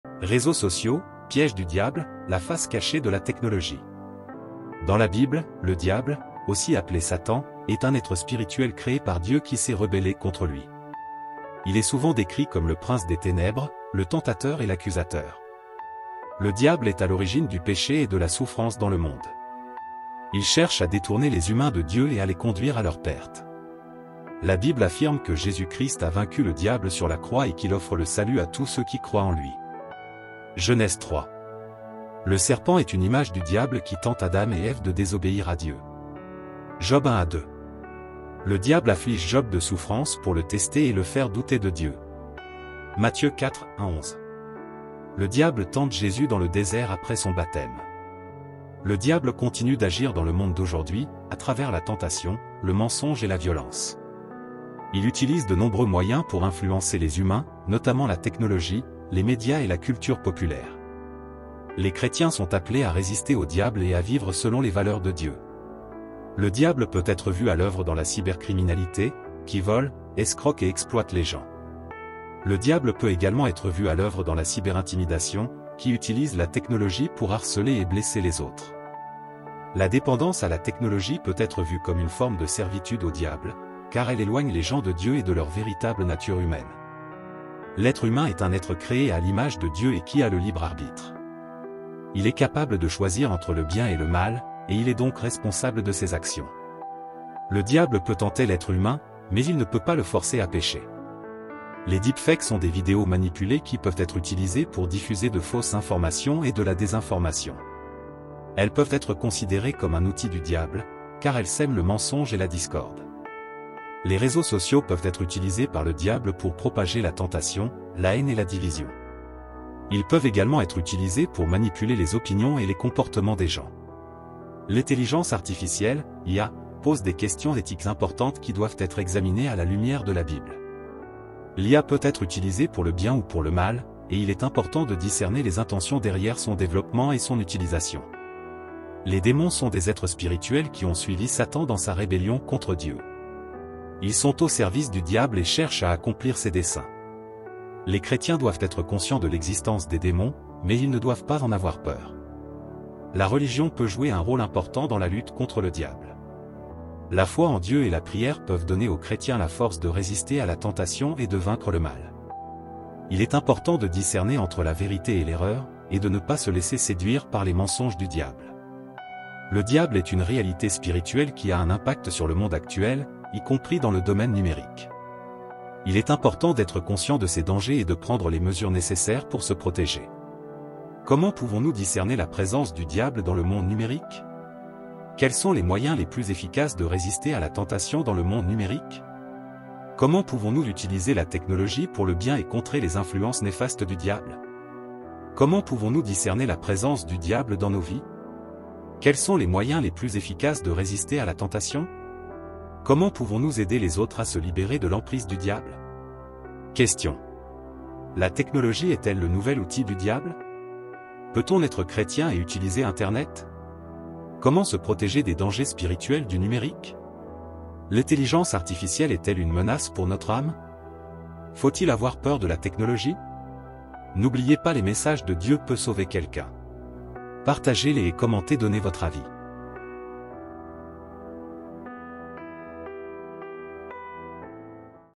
l'IA et Sociaux Pièges du Diable ?
le Diable est une réalité spirituelle qui a un impact sur le monde actuel, y compris dans le domaine numérique.